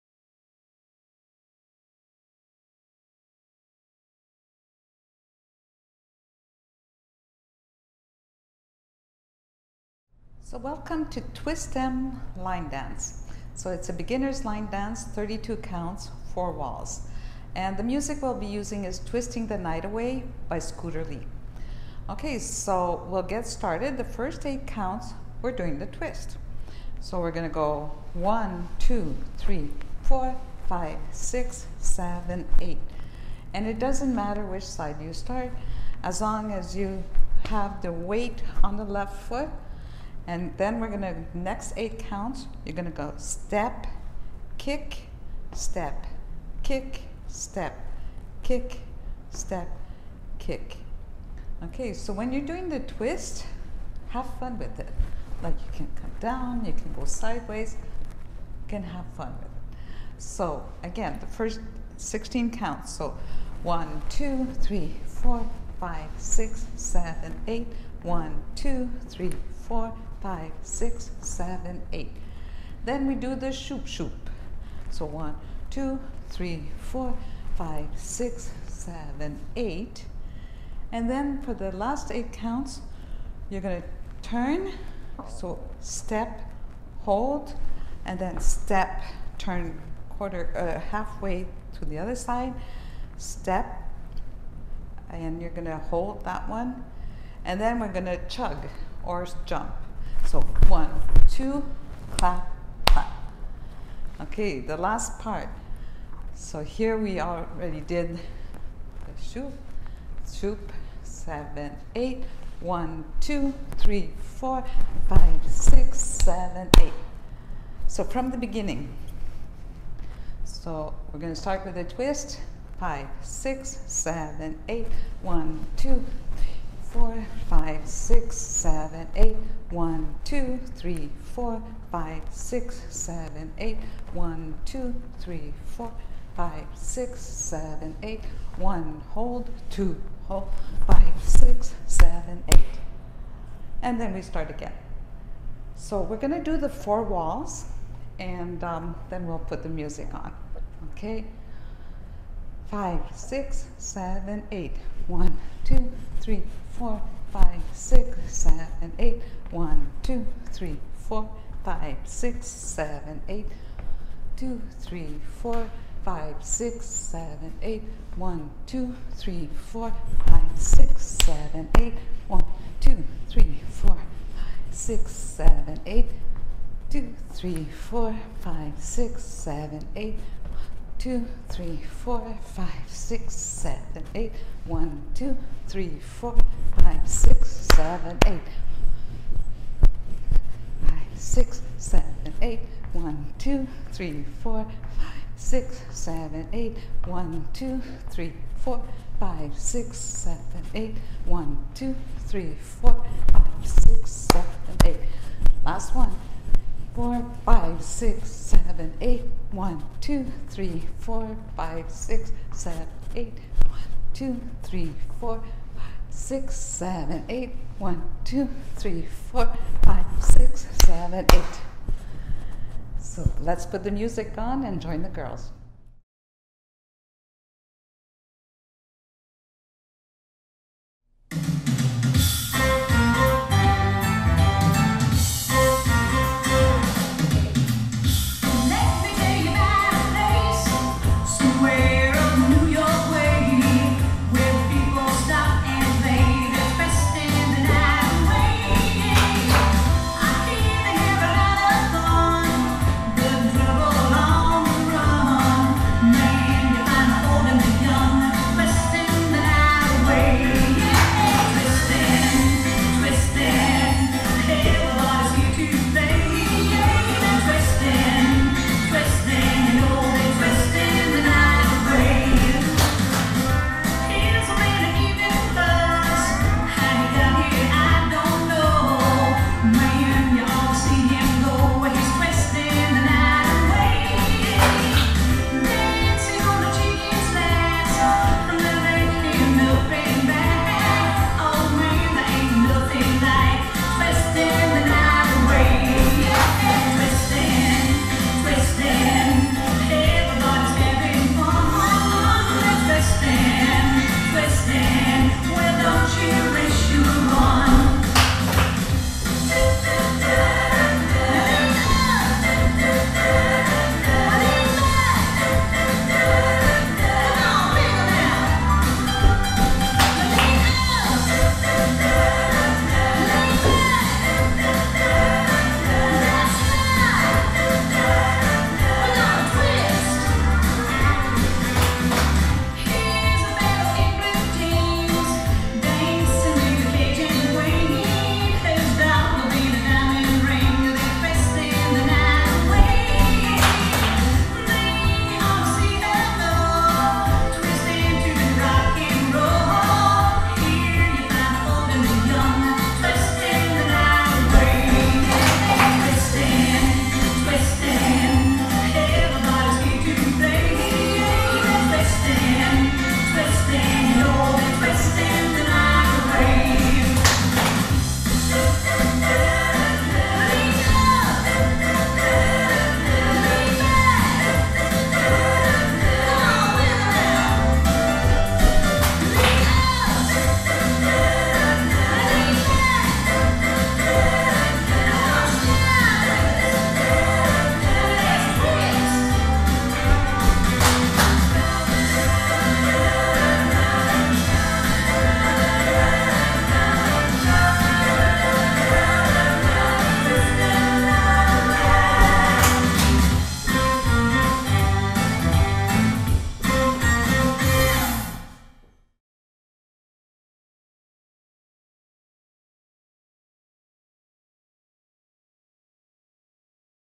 Twist 'Em Line Dance
With Nickie Savage

Beginner - 4 walls 32 count

Choreographer:  Jo Thompson

Dancers:  Nickie Savage / Deb Gallant / Faye Adams

Tutorial then Practice at 3:48